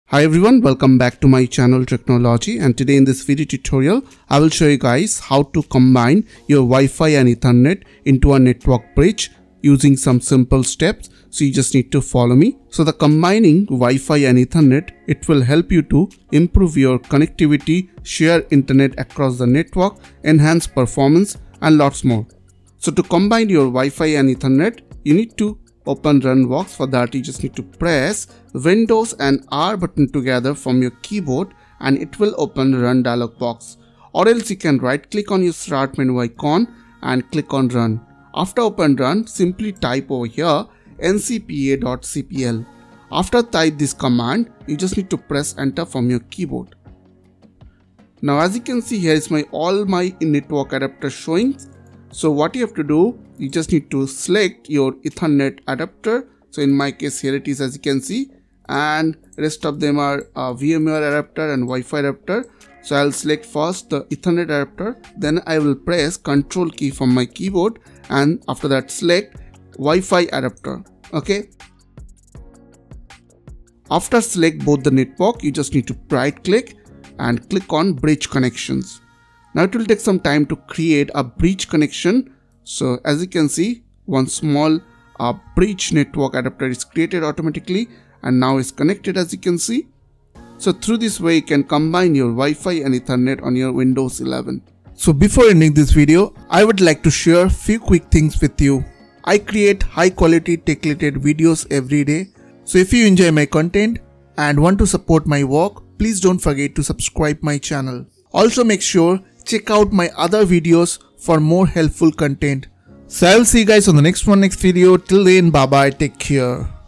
Windows 11: Combine Ethernet and WiFi for MAXIMUM Speed!
Combining Wi-Fi and Ethernet into a network bridge allows seamless communication between devices, extends network coverage, improves connectivity, shares internet access across networks, enhances performance, and enables device interoperability without extra hardware costs.